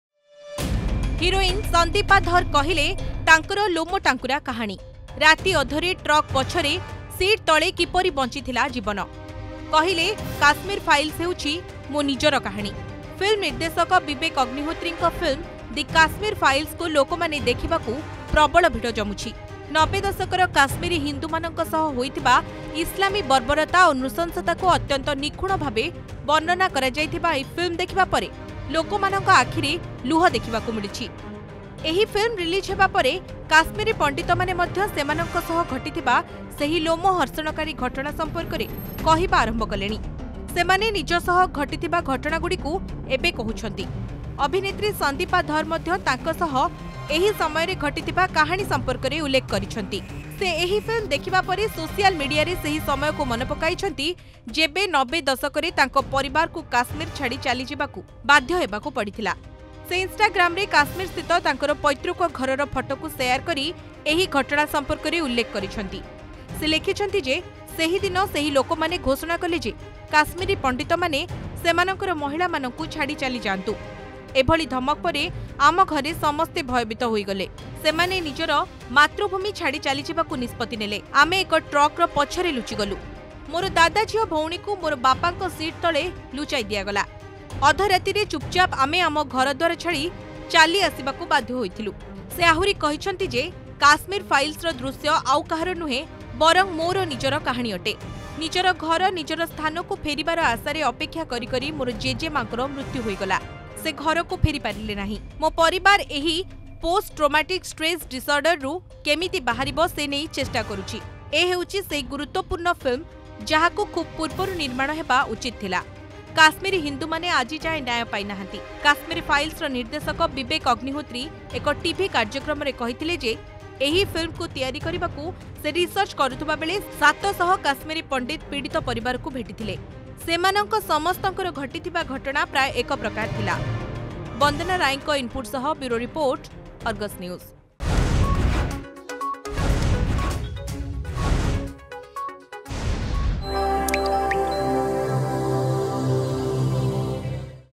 Actress Sandeepa Dhar recalls stories of her leaving Kashmir, Says 'this is literally my own story'
Bollywood Actress, Sandeepa Dhar, whose family had migrated from Kashmir during the Kashmiri Pandit exodus, said that The Kashmir Files reminded her and her family of that dark chapter in their lives and thanked Director Vivek Agnihotri for showing the world the truth.